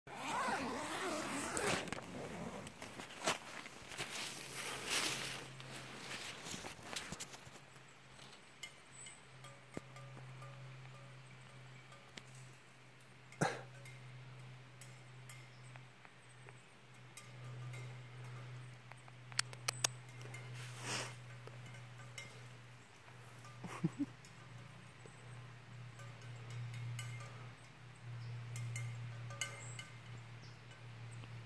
Cowbell Wakeup Call
Woke up to a cowbell this morning.  Looked outside my tent and saw this cutie.  She also visited my site at about 3am :)